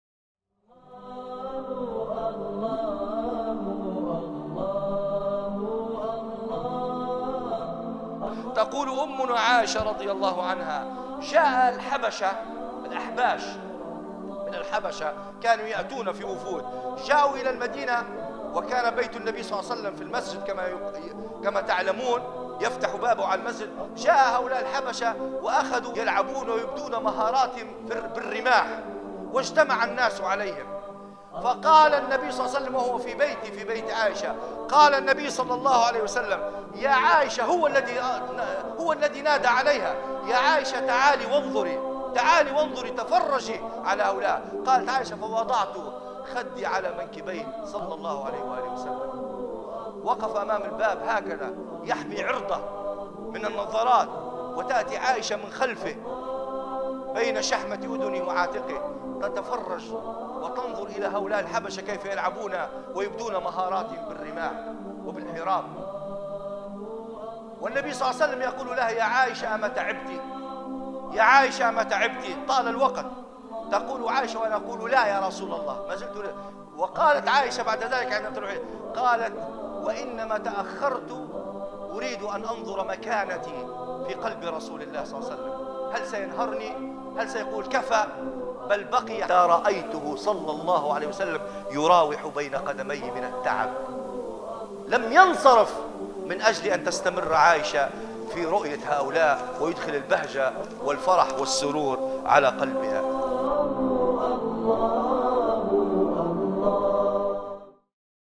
الشيخ عبدالرزاق مشيرب | مؤقف مؤثر للرسول عليه السلام مع عائشة رضي الله عنها
موقف مؤثر للرسول مع عائشة | من أخلاقه صلى الله عليه وسلم | صبر الرسول على زوجاته